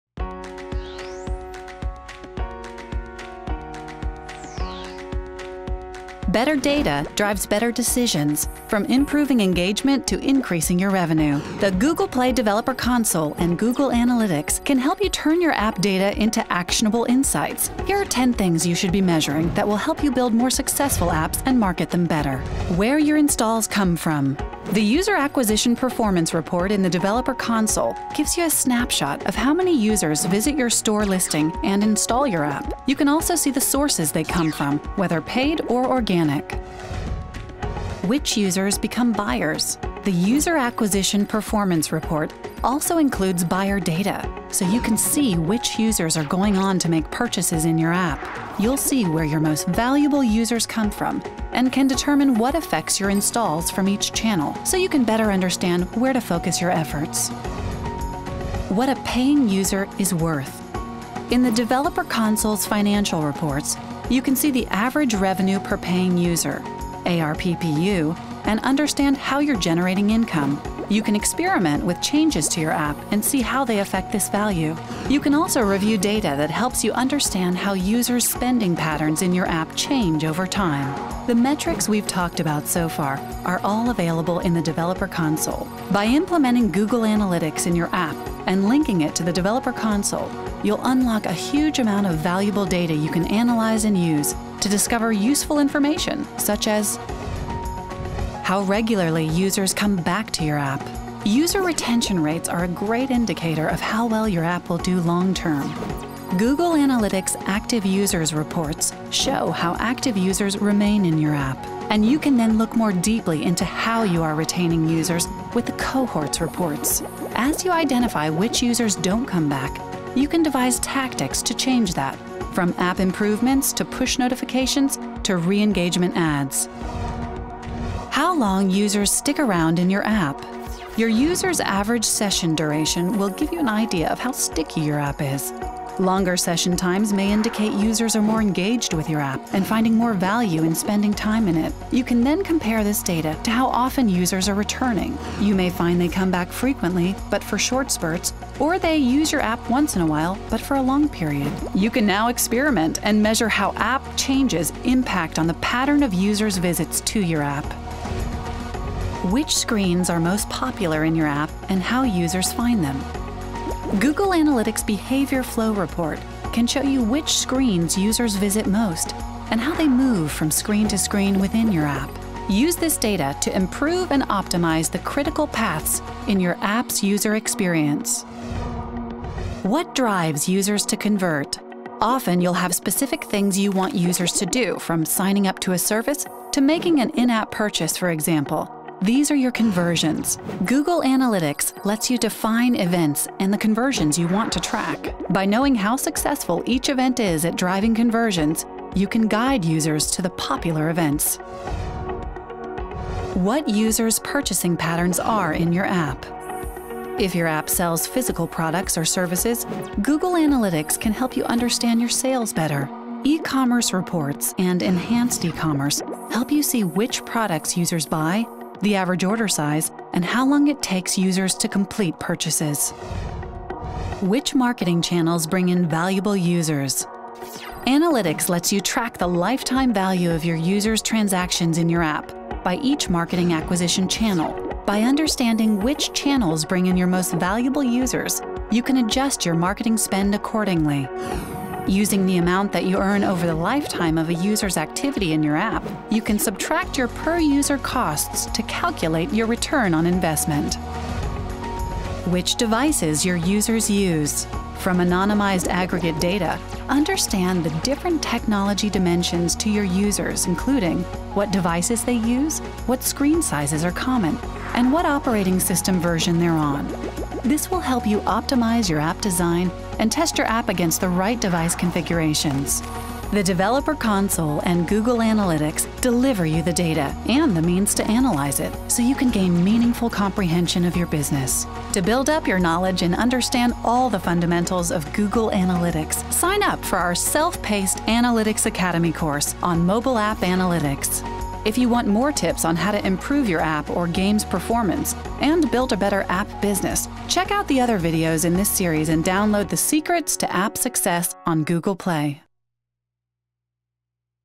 10 things you should be measuring
Google Analytics provides you with a wealth of information about the users of your app, information you can use to enhance your app and its marketing.

In this video you’ll find 10 tips to help you measure the performance of your app:
0:22 — Where your installs come from
0:37 — Which users become buyers
0:59 — What a paying user is ‘worth’
1:38 — How regularly users come back to your app
2:07 — How long user stick around in your app
2:38 — Which screens are most popular in your app and how users find them
2:59 — What drives users to convert
3:22 — What users’ purchasing patterns are in your app
3:43 — Which marketing channels bring in valuable users
4:11 — Which devices your users use